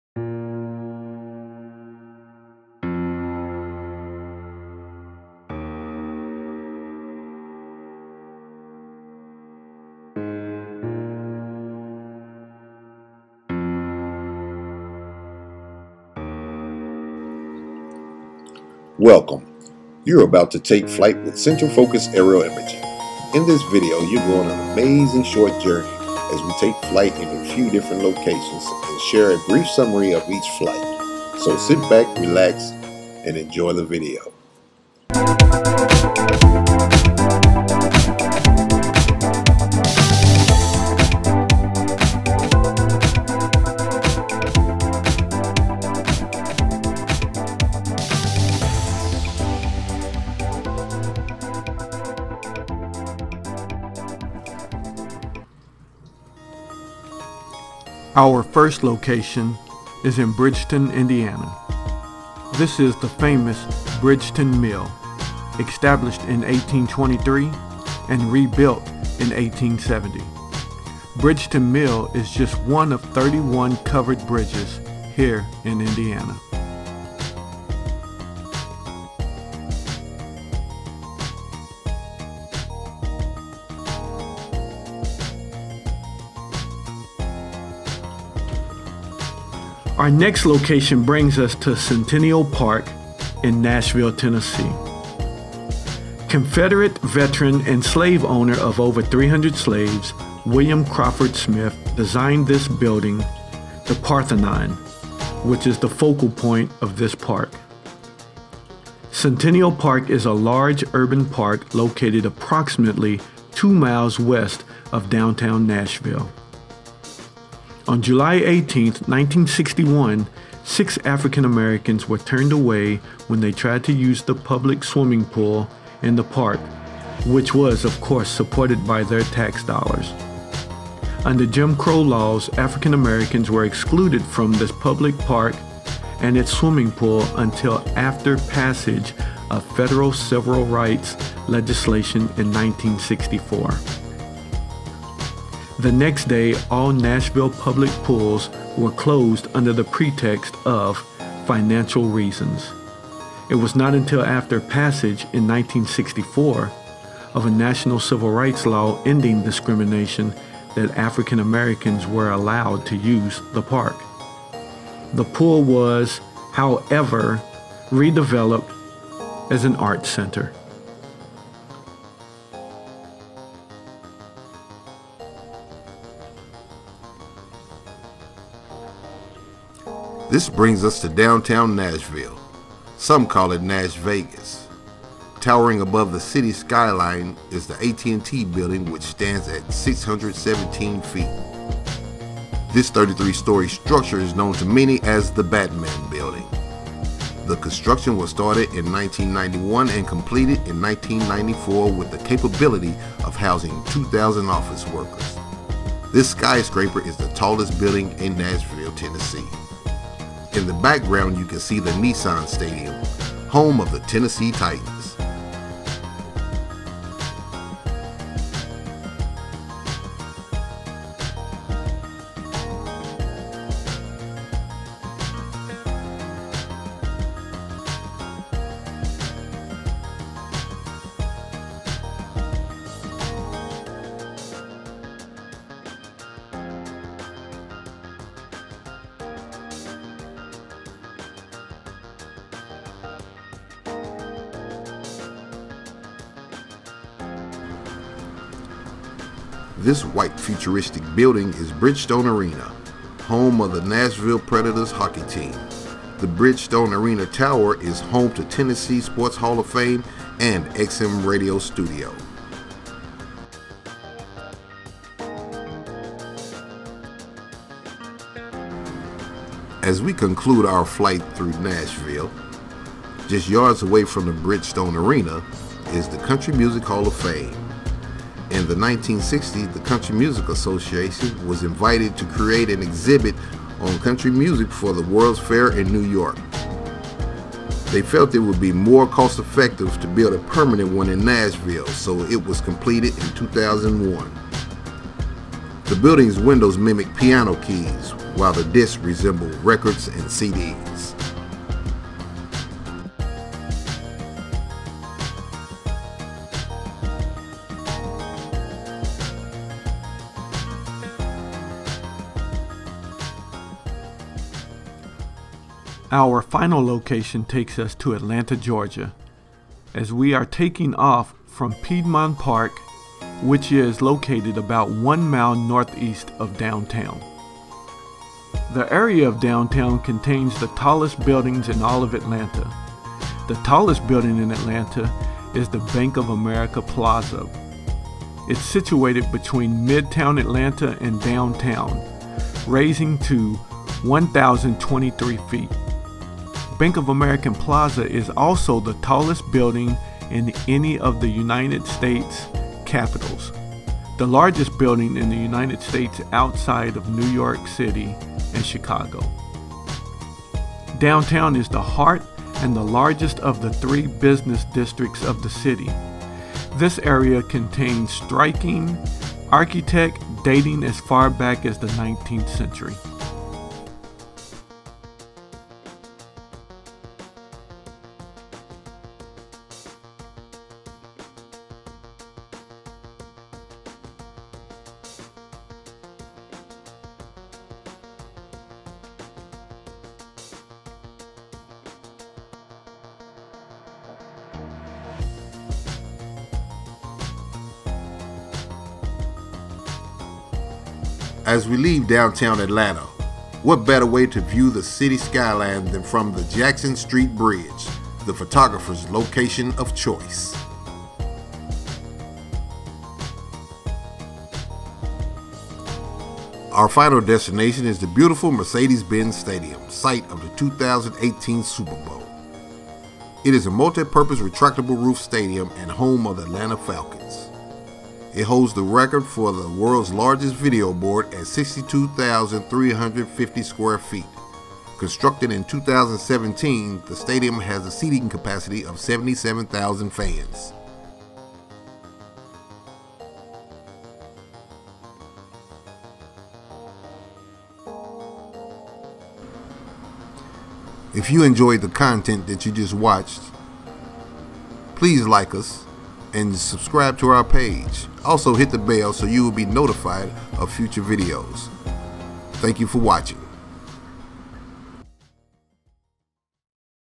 TAKING FLIGHT
Taking a quick look from above as we tell a short story on a few drone outings. We used the Autel Robotics Evo, the DJI 2 Zoom along with the Osmo Pocket.